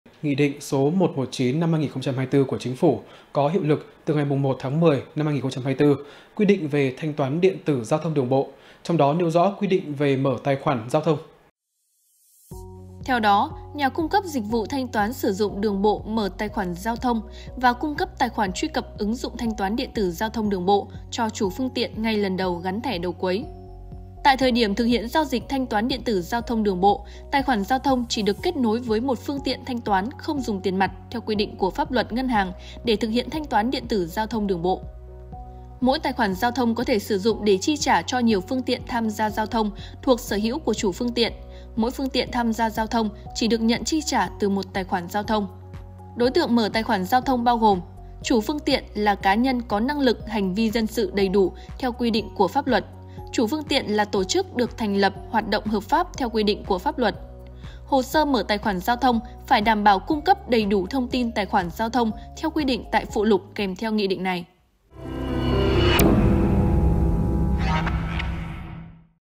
Một số quy định về mở tài khoản giao thông | THLC
Laocaitv.vn - Chính phủ ban hành Nghị định 119/2024/NĐ-CP có hiệu lực từ ngày 01/10/2024, quy định về thanh toán điện tử giao thông đường bộ, trong đó nêu rõ quy định về mở tài khoản giao thông.

Đài Phát thanh - Truyền hình Lào Cai